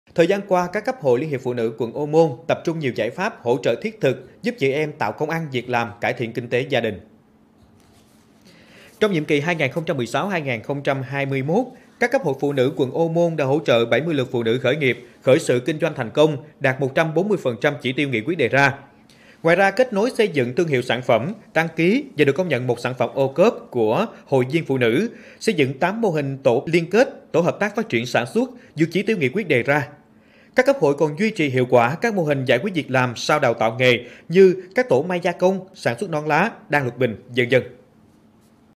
Giúp 70 phụ nữ khởi nghiệp, khởi sự kinh doanh thành công | Cần Thơ TV
Thời gian qua, các cấp Hội LHPN quận Ô Môn tập trung nhiều giải pháp hỗ trợ thiết thực giúp chị em tạo công ăn, việc làm cải thiện kinh tế gia đình.